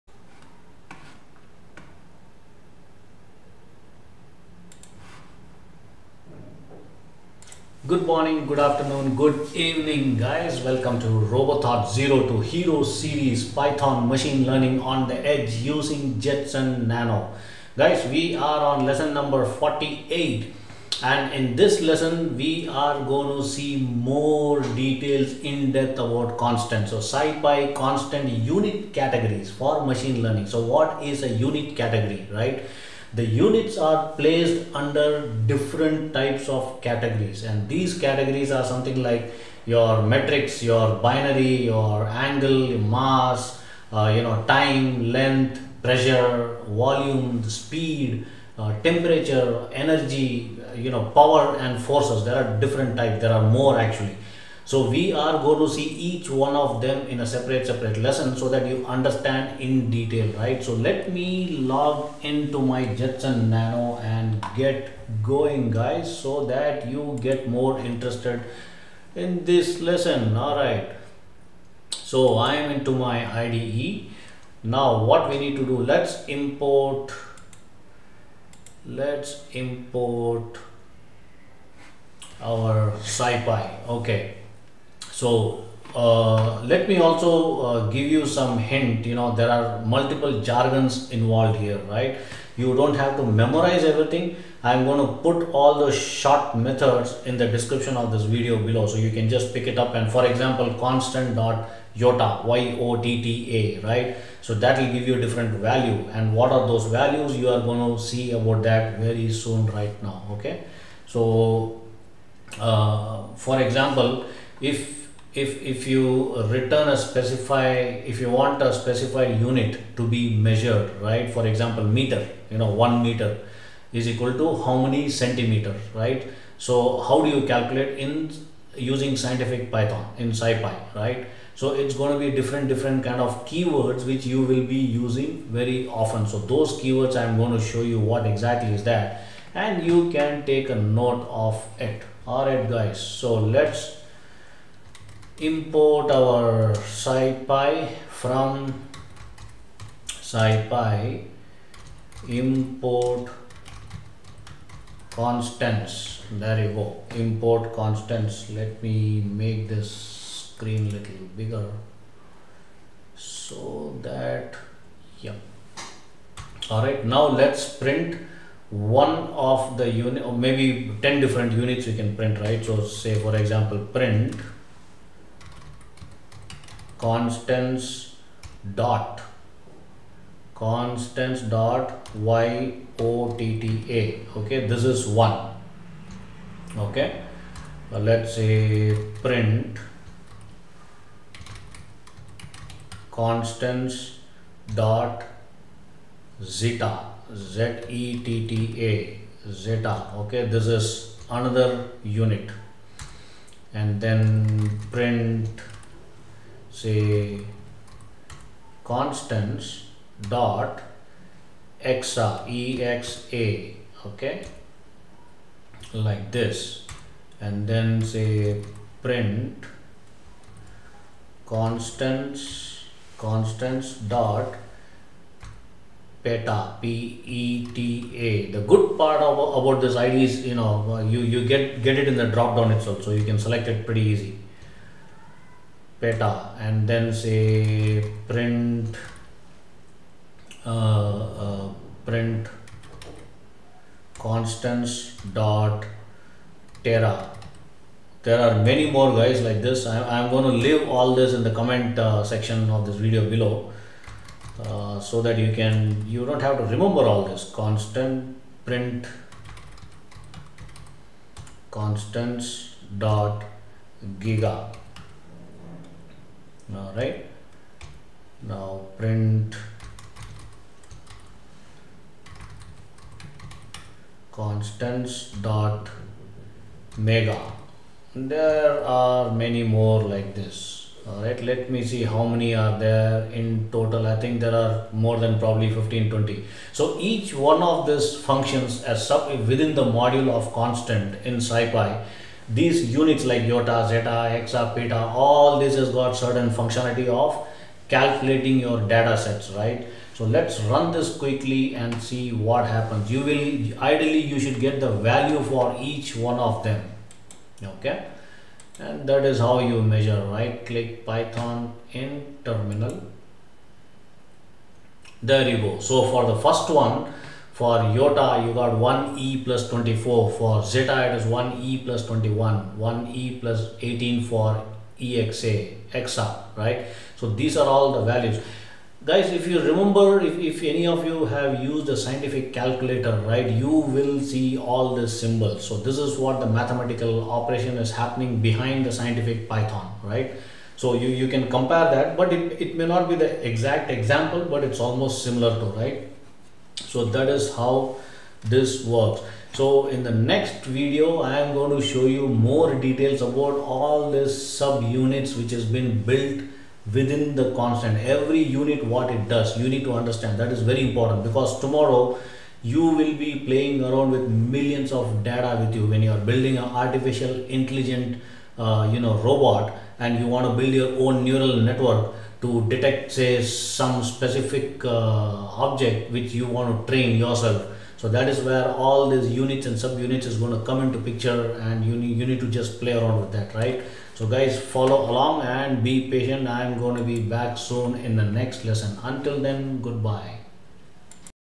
Lesson 48 – SciPy Constant Unit Categories For Machine Learning
The units are placed under the categories like : Metric, Binary, Mass, Angle, Time, Length,  Pressure, Volume, Speed, Temperature, Energy,  Power and Force
Metric (SI) Prefixes:  Return the specified unit in meter (e.g. centi returns 0.01)

Example
from scipy import constants